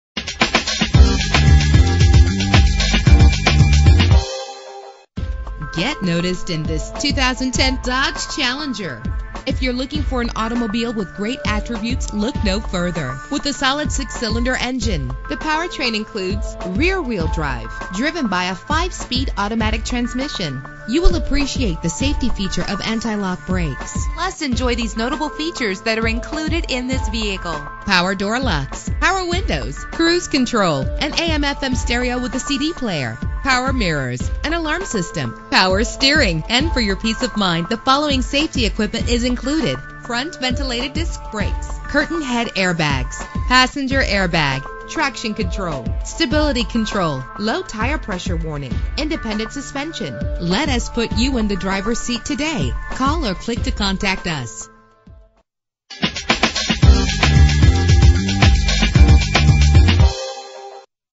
2010 Dodge Challenger - Lewisville TX
Year: 2010
Make: Dodge
Model: Challenger
Trim: SE
Engine: 3.5 liter 6 cylinder 24 valve
Transmission: 5-Speed Automatic
Color: Brilliant Black Crystal Pearlcoat
Mileage: 47607
Address:
1024 South I-35 East, Exit 450